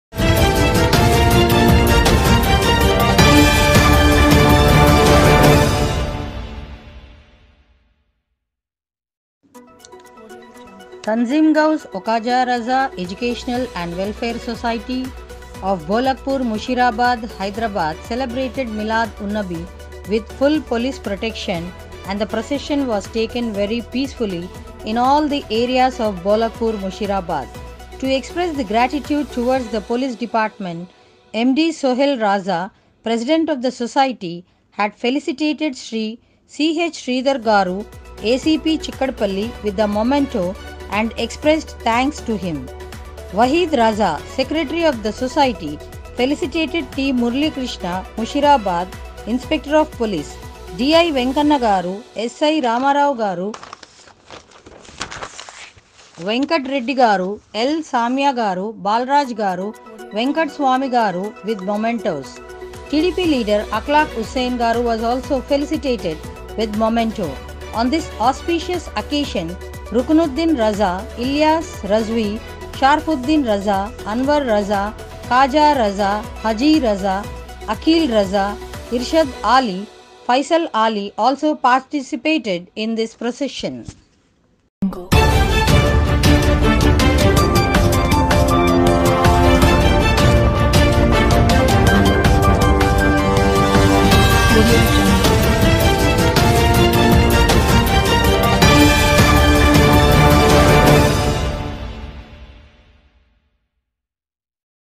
BTS NEWS TANZEEM GHOUSE O KHAJA RAZA EDUCATIONAL &WELFARE SOCEITY BHOLAKPUR MUSHEERABAD HYD KI TARAF
TANZEEM GHOUSE O KHAJA RAZA EDUCATIONAL &WELFARE SOCEITY BHOLAKPUR MUSHEERABAD HYD KI TARAF SE EID MILAAD UNNABI SALLALAHUALAHIWASSALAM KE MAUQE PAR BHARI PEACE FUL BANDHU BAST KARNE PAR
MD SOHAIL RAZA
PRESIDENT TGKR NE SRI CH SRIDHAR GARU ACP CHIKKADPALLY KO MOMENTO PESH KIYA AUR GULPOSHI BHI KI .MUSHEERABAD INSPECTOR OF POLICE JANAB T MURALI KRISHNA GARU KO BHI MOMENTO   WAHEED RAZA SECRATERY TGKR NE PESH KIYA .AUR  VENKANNA GARU  (D.I)   RAMARAO GARU  (S.I)
VENKAT REDDY GARU (S.I)
 L.SAMYA GARU (S.I)
BALRAAJ GARU (S.I)VENKAT SWAMY GARU (S.I)
MUSHEERABAD KO BHI MOMENTO PESH KIYA GAYA AUR SHUKRIYA ADA KIYA GAYA .AQLAQ HUSSAIN SB TDP LEADER KI GULPOSHI KIGAYI AUR SHUKRIYA ADA KIYA GAYA .ISMAUQE PAR RUKUNUDDIN RAZA  (TREASURER)
ILYAS RAZVI
AUR SHARFUDDIN RAZA
ANWAR RAZA KHAJA RAZA HADI RAZA AQEEL RAZA IRSHAD ALI  FAISAL ALI MAUJOOD THAY.